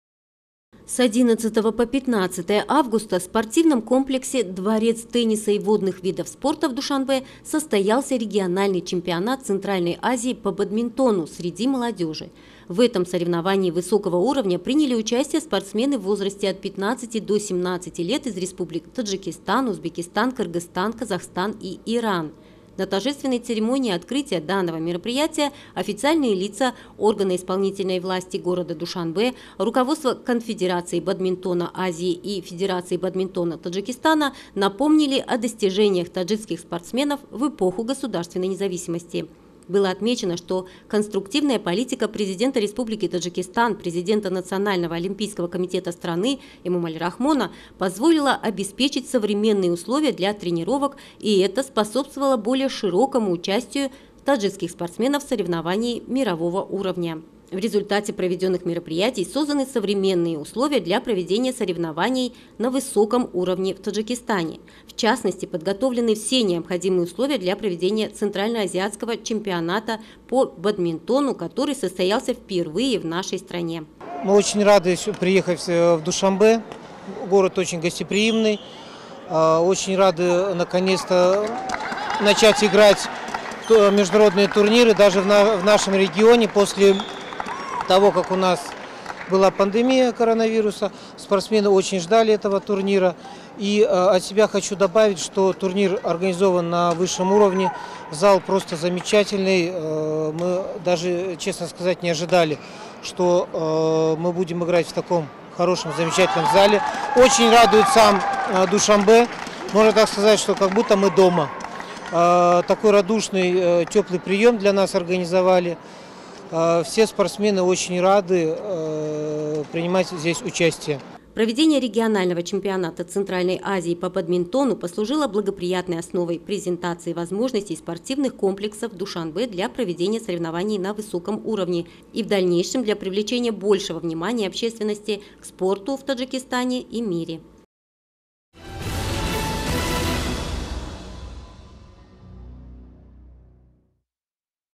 Впервые в Душанбе проведен международный турнир по бадминтону
С 11 по 15 августа в спортивном Комплексе «Дворец тенниса и водных видов спорта» в Душанбе состоялся Региональный чемпионат Центральной Азии по бадминтону среди молодежи   в Душанбе. В этом соревновании высокого уровня приняли участие спортсмены в возрасте от 15 до 17 лет из республик Таджикистан, Узбекистан, Кыргызстан, Казахстан и Иран.